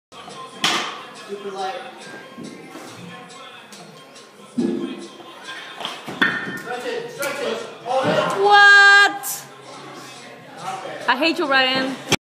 70kg power snatch bw 59kg 56kg weightlifter
70kg power snatch with straps on 2/5/13 compete in the 56 kg weight class